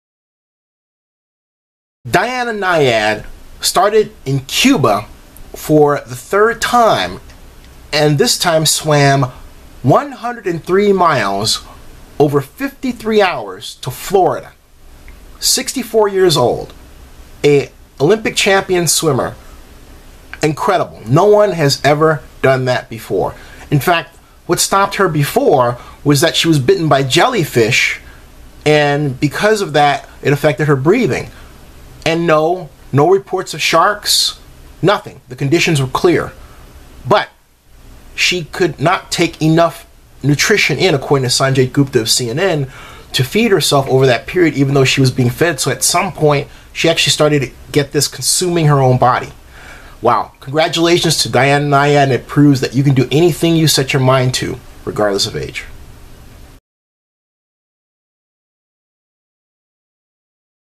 Diana Nyad First To Swim From Cuba To Florida
Zennie62 - vlog produced by Zennie Abraham Zennie62 Blog - youtube video